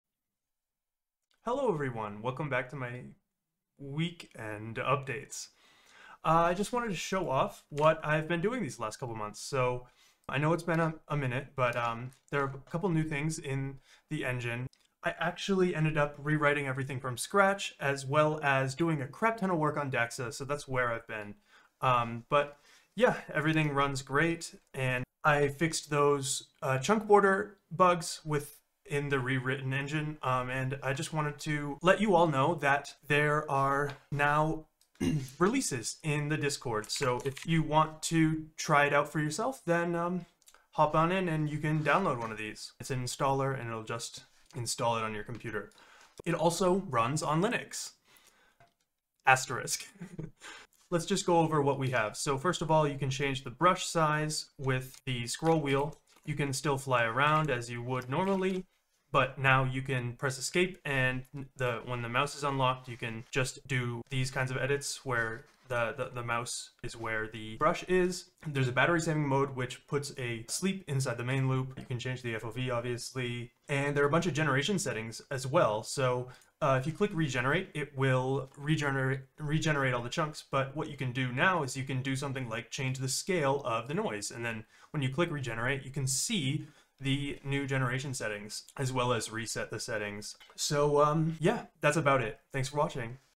I Created an Installer for my Voxel Engine! | Voxels *Weekend update #12
Hey everyone! I just wanted to put out a video to confirm I'm still making some progress on my engine, though some complications have been stunted a bit with some other things I'm working on!

You can download the release without joining my Discord by going to the releases on GitHub:

GPU: RTX 3070 (8GB VRAM)
CPU: AMD Ryzen 7 5800x (8 core/16 thread)
RAM: 16GB 2667 MHz DDR4